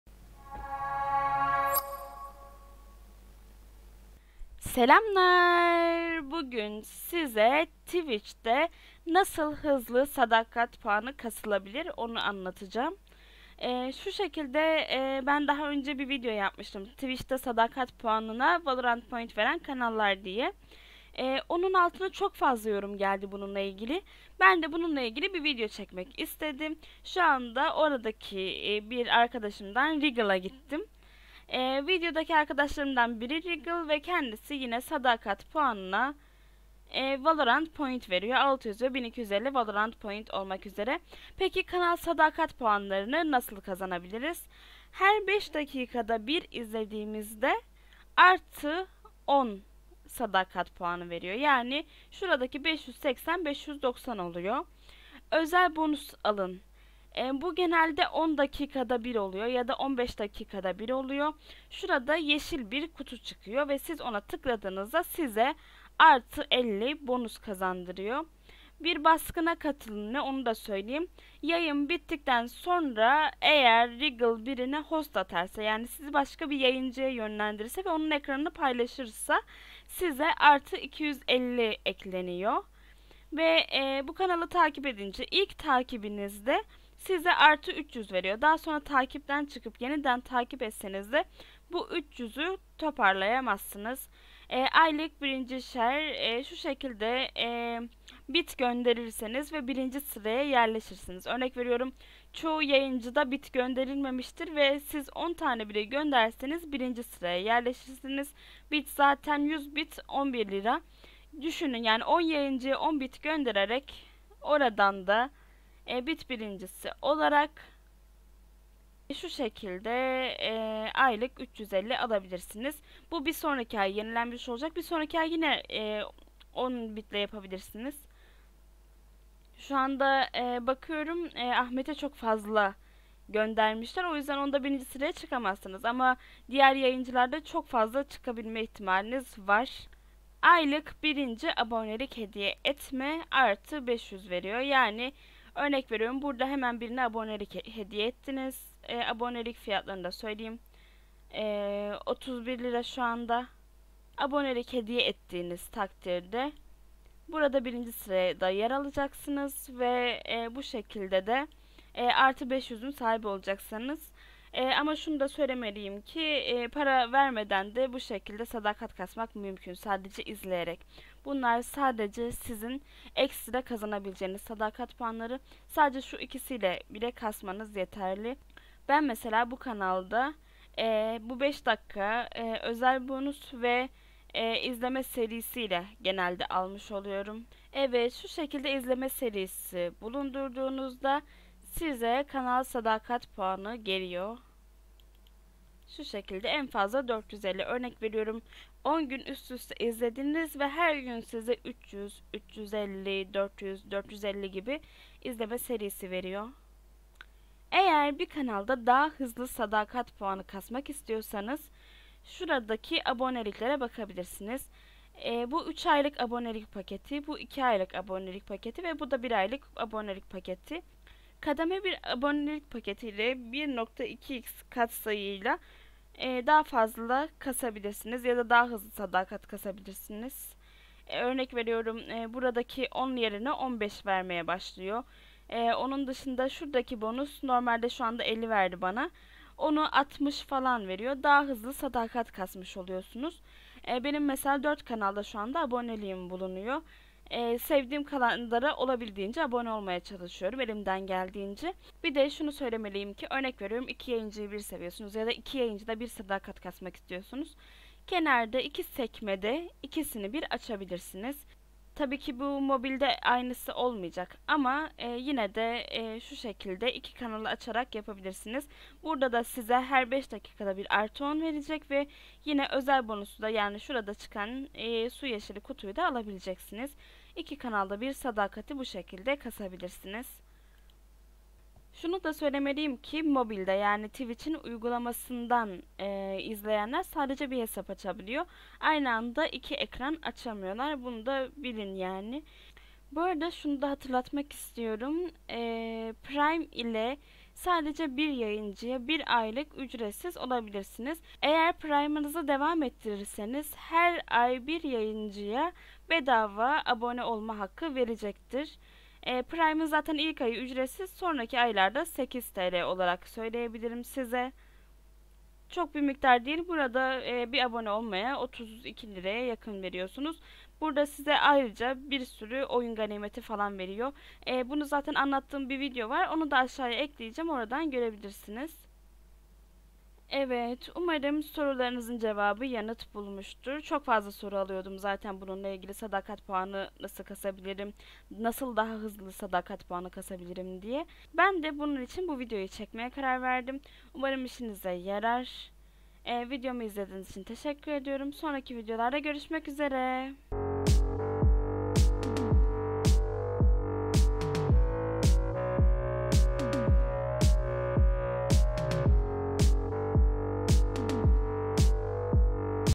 Twitch'de sadakat puanı nasıl kasılır? İşte Twitch'de hızlı sadakat puanı kasmanın yolları
Selamlaar,

Bugün size Twitch'de hızlı sadakat kasmanın yollarını anlattım. Bir önceki videoma çok yorum gelmişti. Ben de bunun için bu videoyu çektim.

-Düzeltme-
Kademeler aylık olarak değişiyor yani Kademe 3 de 1 aylık Kademe 1 de 1 aylık fakat getirdiği XP değişiyor.